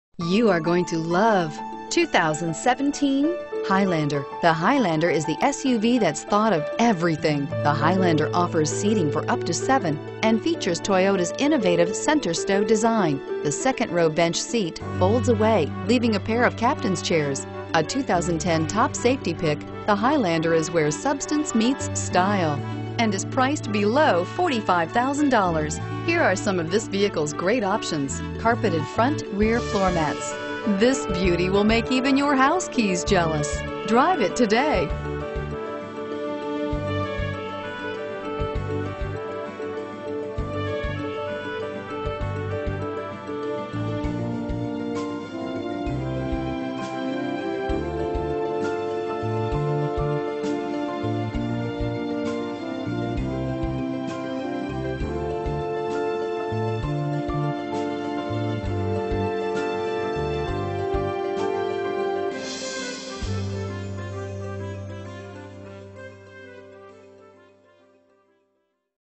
2017 Toyota Highlander Elk Grove Toyota 116217
2017 Toyota Highlander  - Stock#: 116217 - VIN#: 5TDDZRFH8HS454613

Check out this 2017 Toyota Highlander.   Featuring 8-speed Automatic transmission, it has 0 miles.  Contact the dealership today.